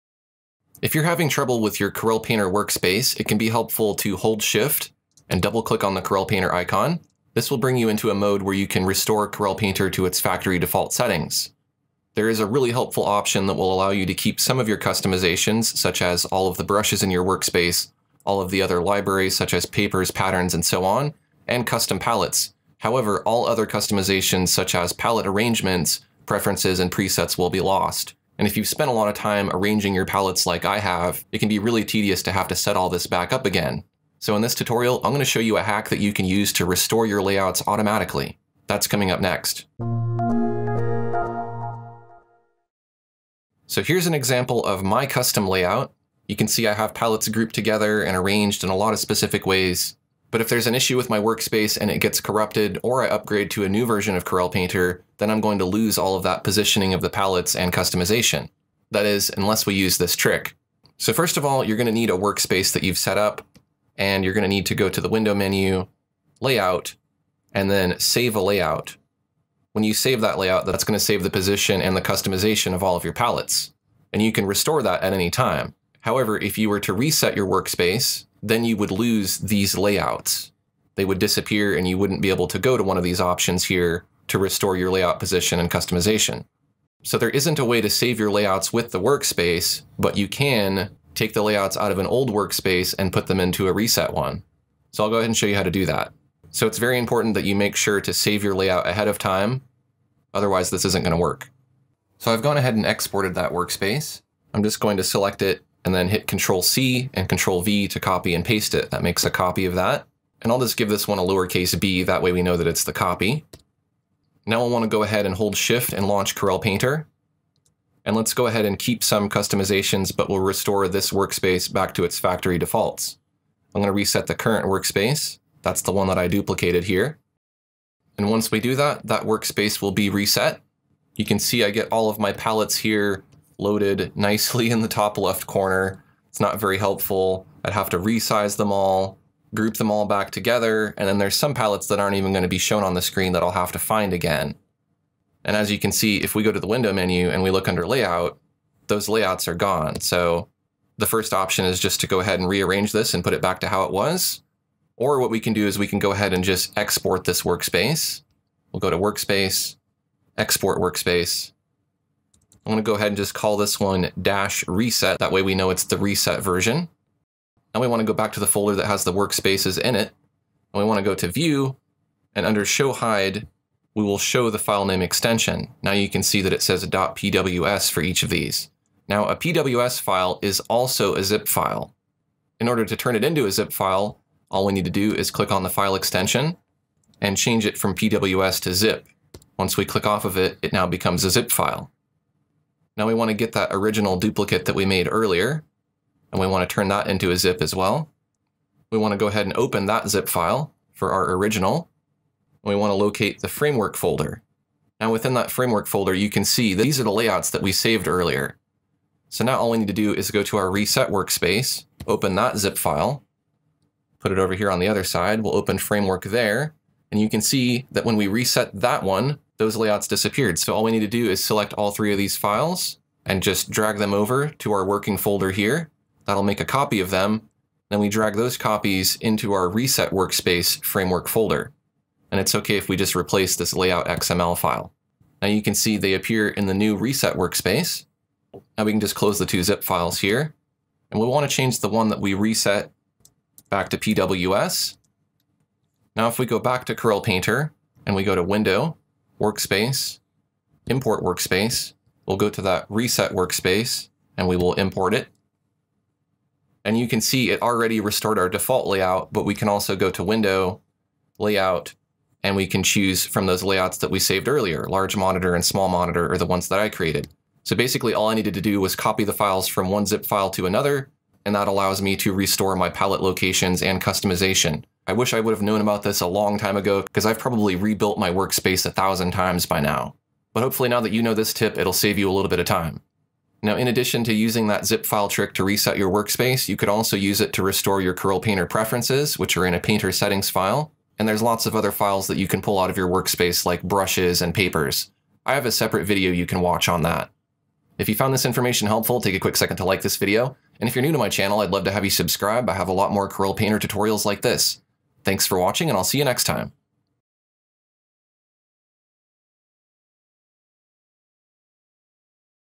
Restore Palette Locations After Factory Reset - Corel Painter Tutorial
Sometimes it can be necessary to reset your workspace to factory defaults, but once you do that, you lose the arrangement and customization of your layout. That is until now. This simple workspace hack will automatically restore your palette customization even for a reset workspace.